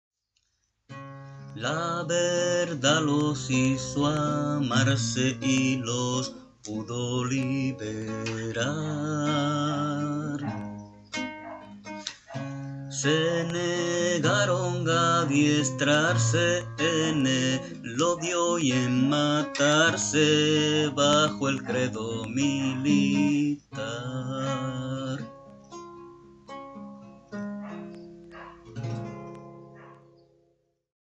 La Verdad los hizo libres
La Verdad los hizo amarse
y los pudo liberar...
Se negaron a adiestrarse
en el odio y en matarse
bajo el credo militar.

Jesús María Bustelo Acevedo